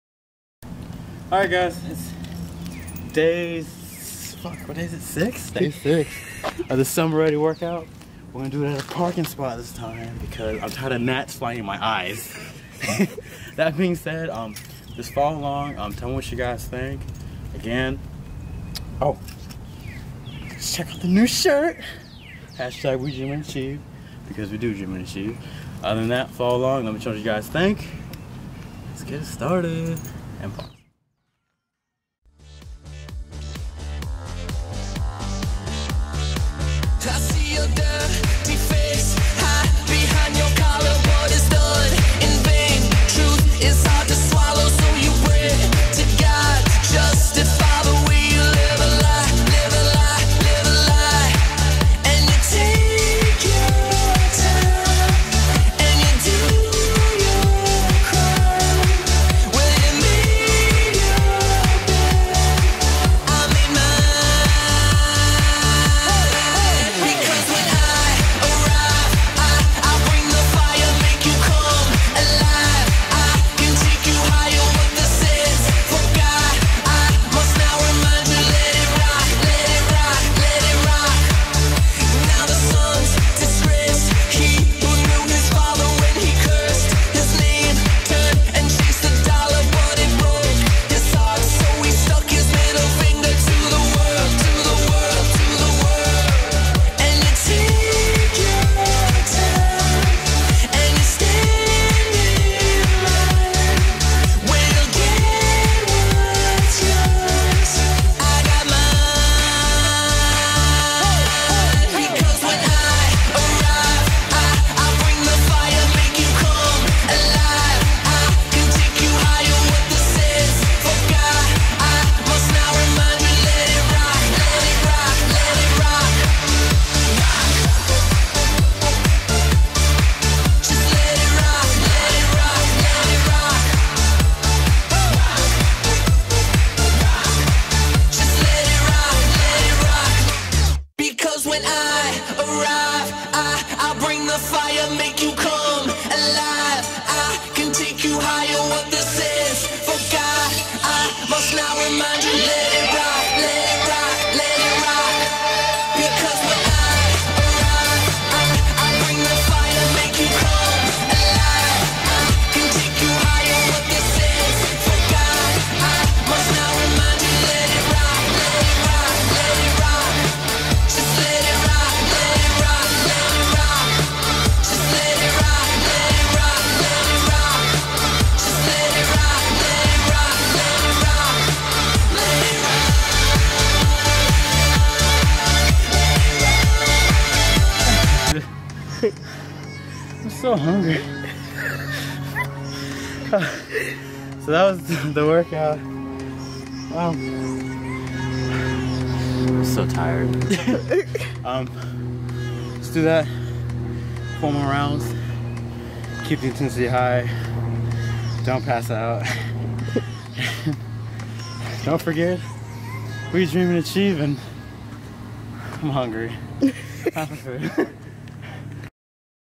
DreamerPat's Summer Ready Workout Day 6
Here is another Summer Ready Workout for everyone. as always 4 rounds of everything and dont forget to keep the intensity high